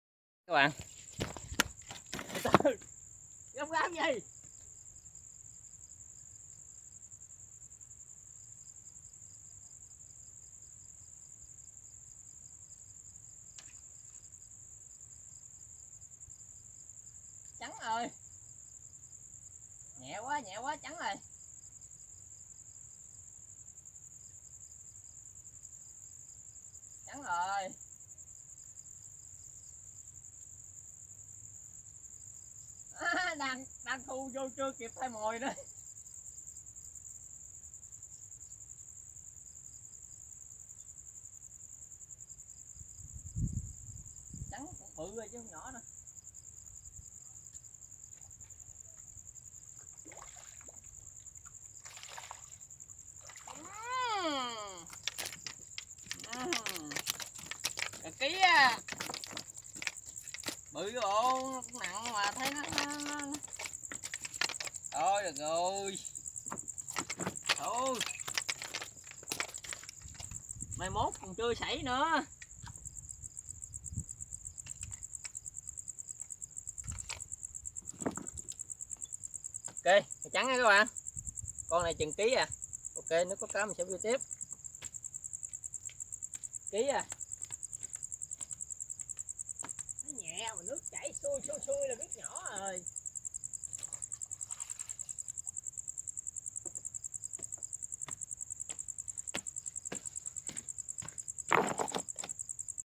Câu Mè Đêm Hồ Gia Ui Bằng Mồi Khoai Lang ủ Chua! #72
Câu Cá Mè Đêm Hồ Gia Ui Bằng Mồi Khoai Lang ủ Chua!
Làm Mồi và đi câu câu cá anh em ủng Hộ Mình nhé
Giúp mình Đạt 100k subscrib nhé

👉Bản Quyền thuộc về Đam Mê Câu Cá OFFICIAL
👉Copyright by Đam Mê Câu Cá OFFICIAL "Do Not Reup"